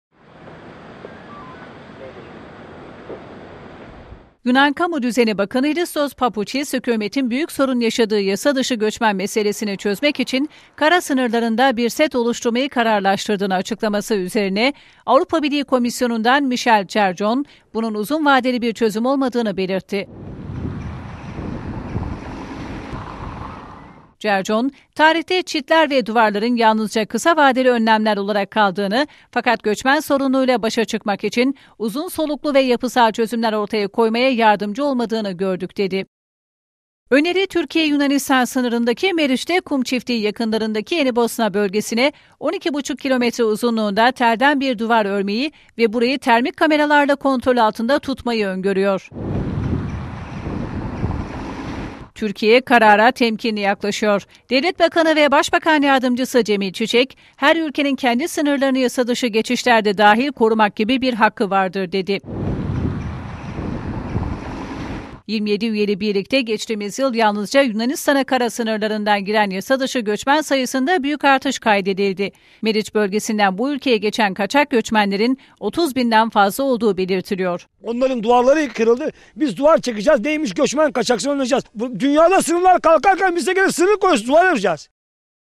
Yunan sınırlarına tel örgü 05.01.2011
Yunanistan sınırları kapanıyor
TEL ÖRGÜDEN DUVAR BRÜKSEL'DEN KABUL GÖRMEDİ